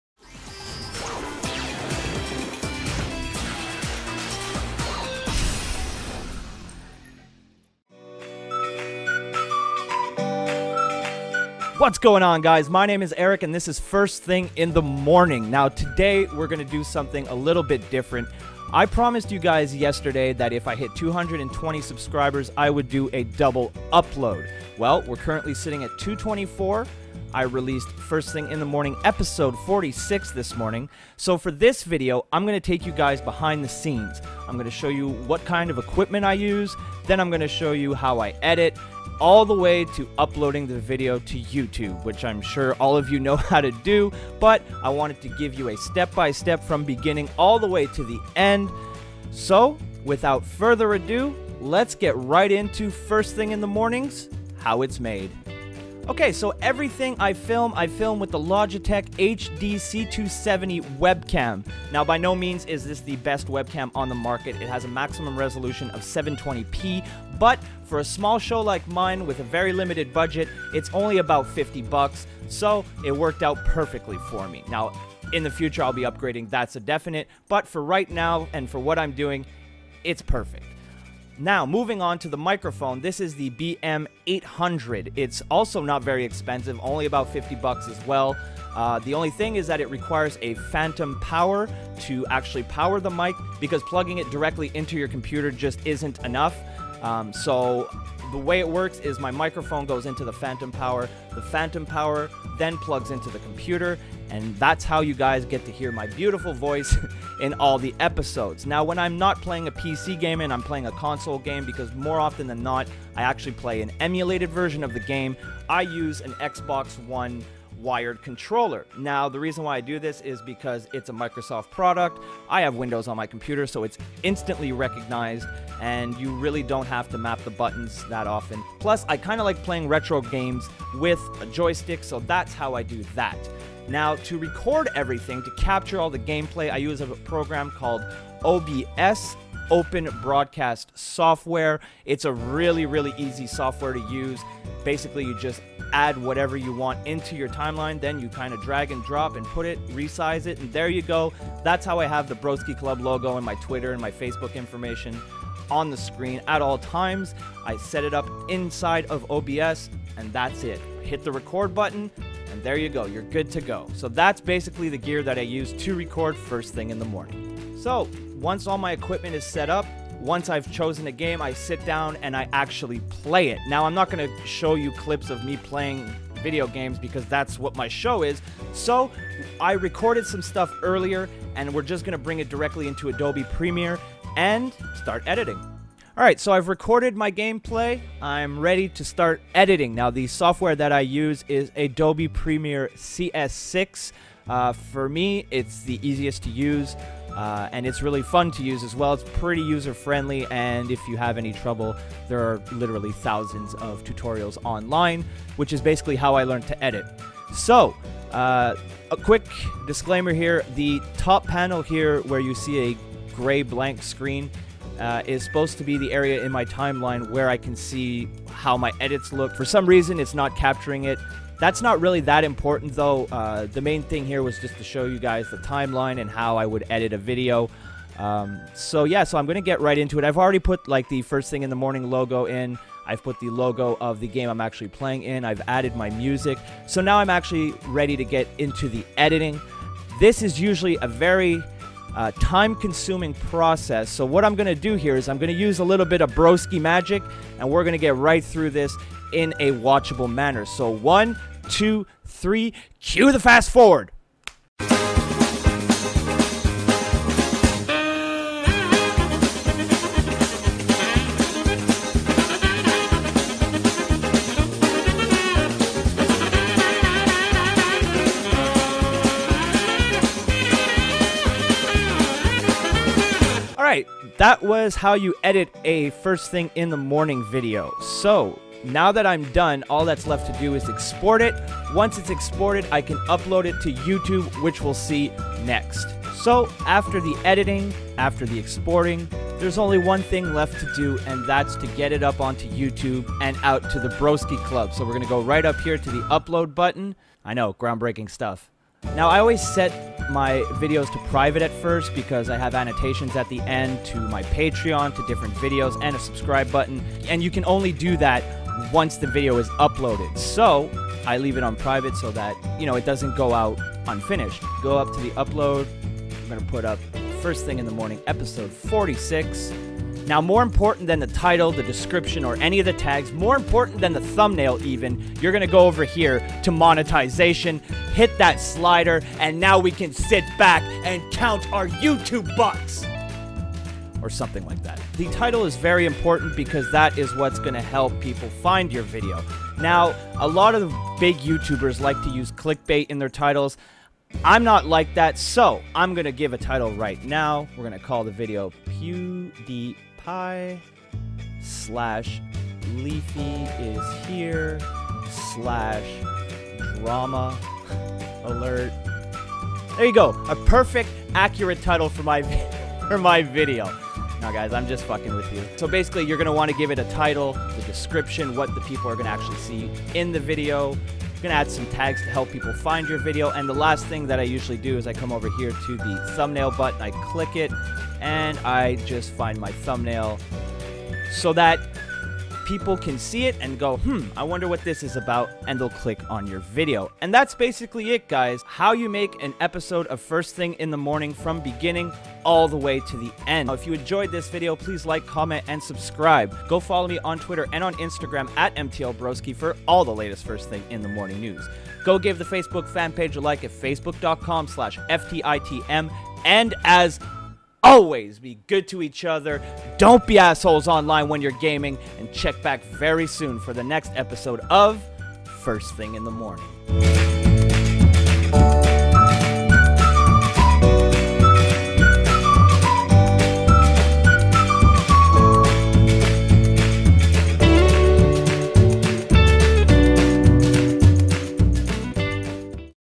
How It's Made - Episode 46 of FTitM - 200 SUBSCRIBER THANK YOU part 2!
This time I take you guys behind the scenes and show you exactly how FTitM is made!